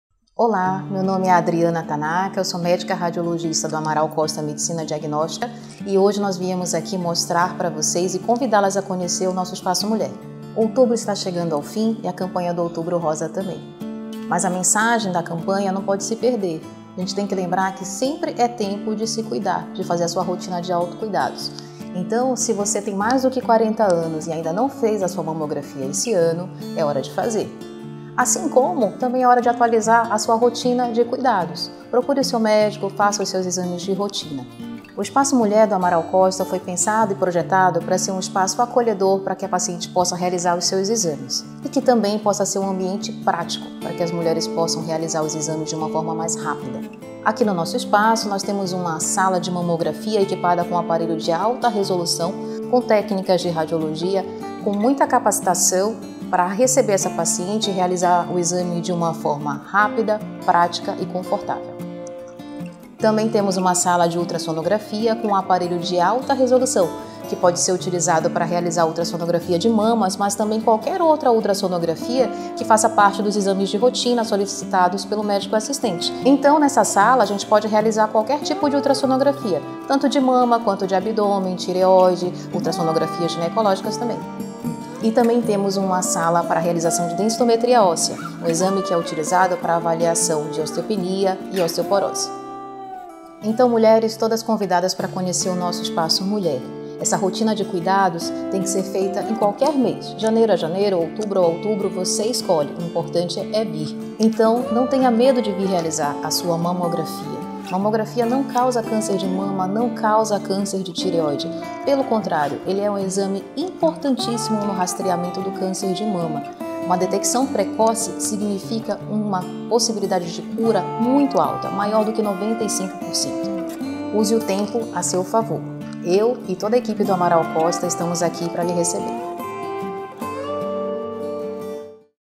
SAÚDE ALÉM DO OUTUBRO ROSA | CONHEÇA O ESPAÇO MULHER
Outubro chega ao fim, mas a saúde da mulher é importante o ano inteiro. Neste vídeo, a Dra. Adriana Tanaka, médica radiologista, apresenta o Espaço Mulher. Um cantinho especial para você realizar seu exames de rotina no Amaral Costa, como mamografia, ultrassonografia e densitometria óssea. Tudo com tecnologia, conforto e segurança. Previna-se com seu médico. Cuide-se com o Amaral.